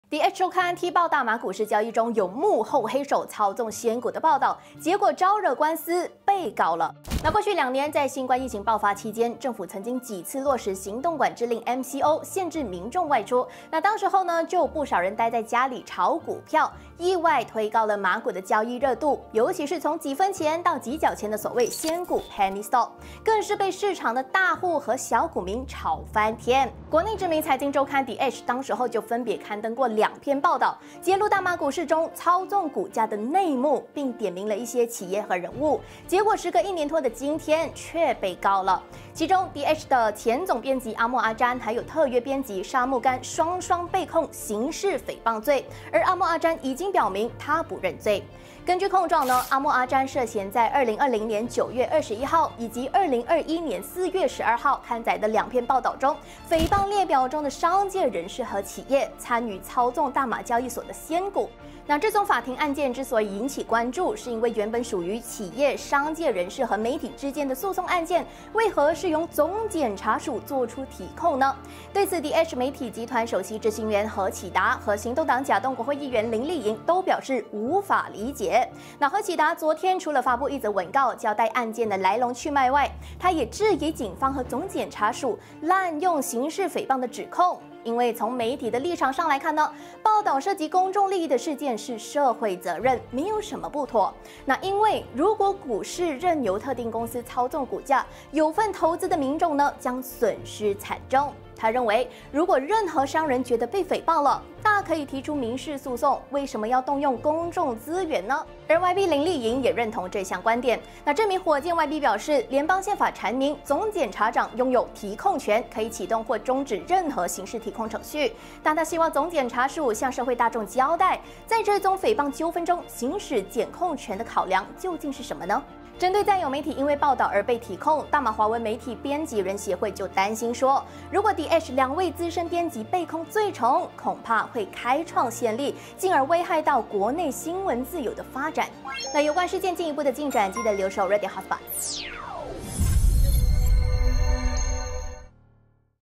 The Edge踩到红线？踢爆仙股操作内幕
▌热点短视频 ▌ 马股版本的“仙股风暴”有新发展，这一回是踢爆“内幕”的The Edge财经周刊，被告刑事诽谤。不过，由总检察署“开这一枪”行驶提控权，就为案件添加了想象空间！

热点站主 | @洪夏诗